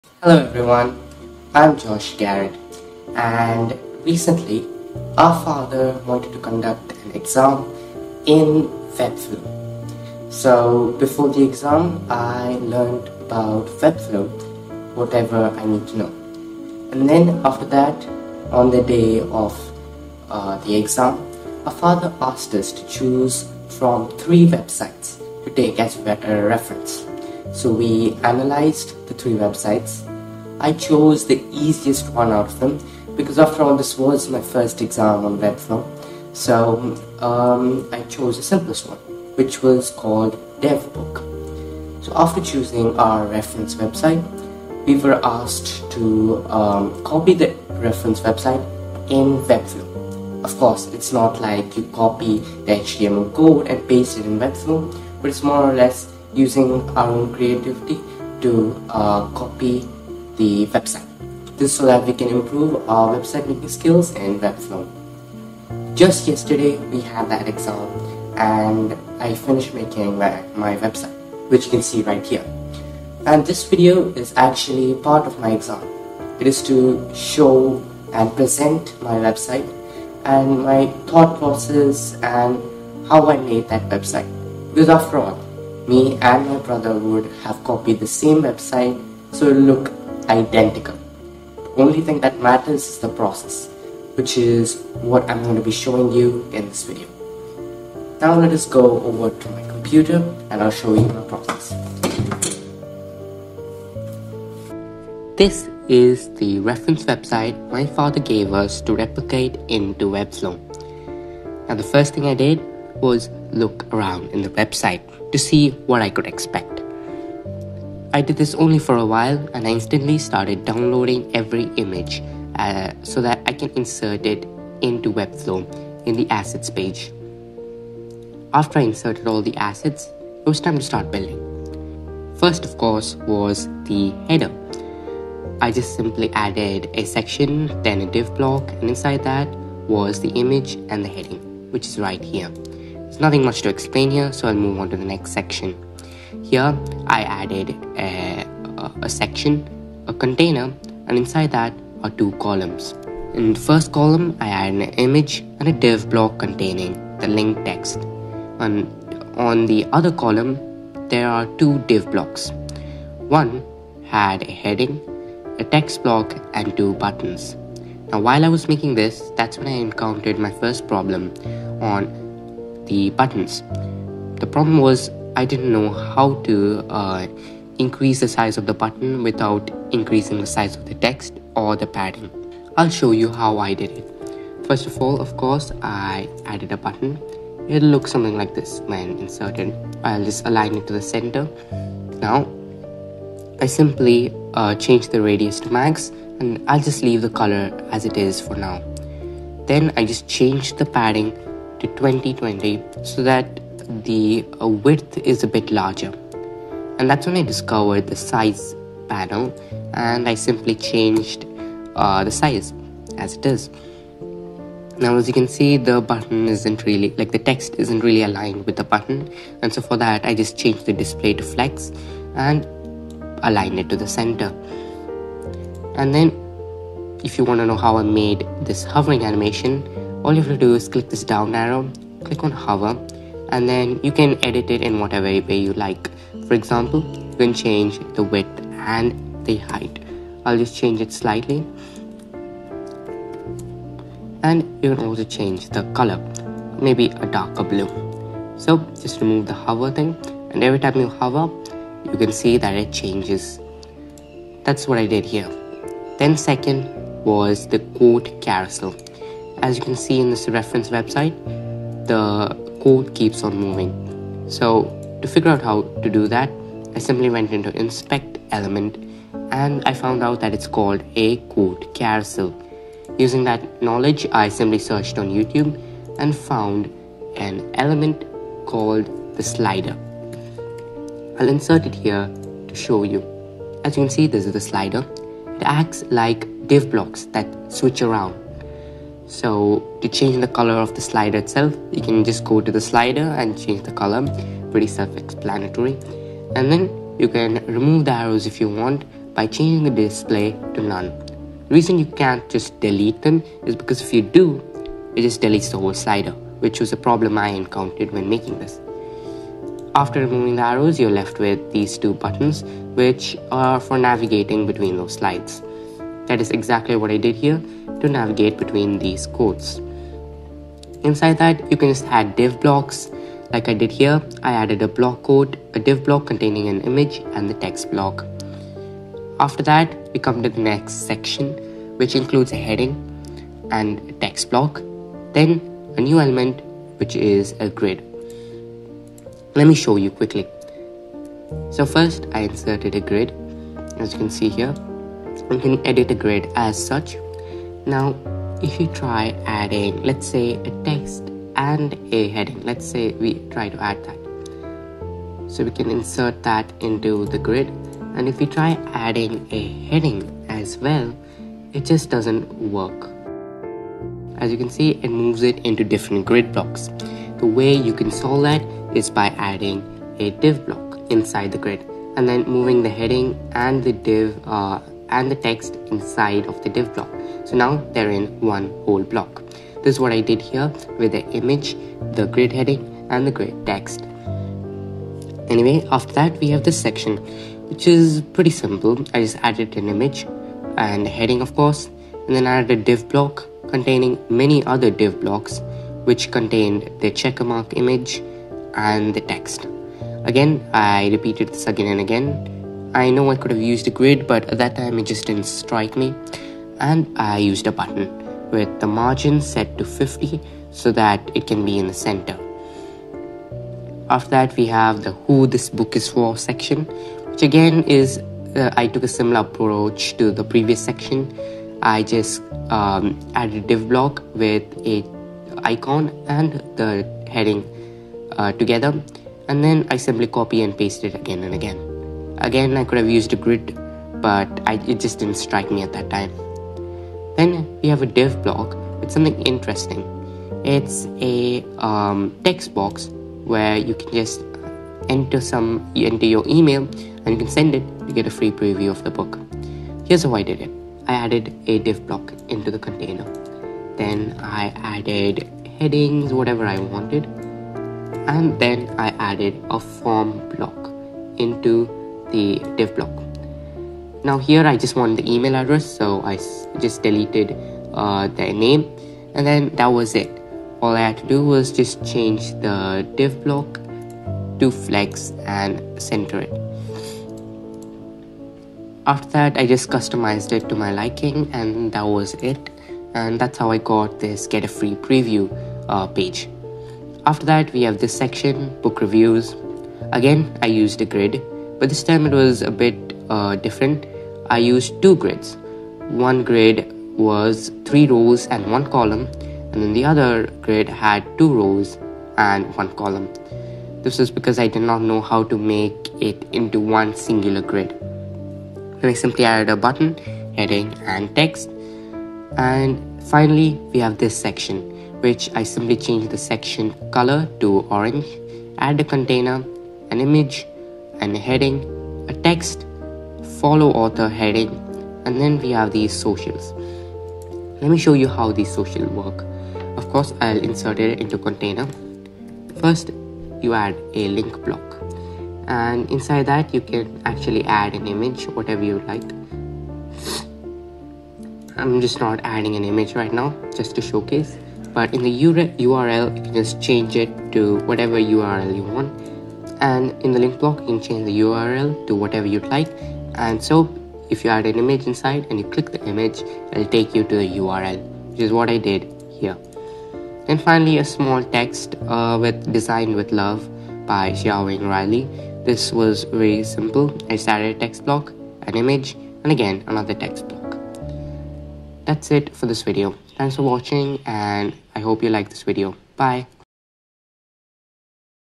How I made my website in my Webflow exam! - Website link in description!
As you know we are homeschooled. So our father conducted a web flow exam for us to test our skills. Now I would like to tell you that everything I showed you in this video are all learnt in the exam. So they might not be always right.

(It might be bugged on devices smaller than laptops as I haven't made the website responsive.)

🛠️Tools used in the making of the video:
Thumbnail: Pixlr
Editing: Wondershare Filmora 12
Recording: Google Pixel 4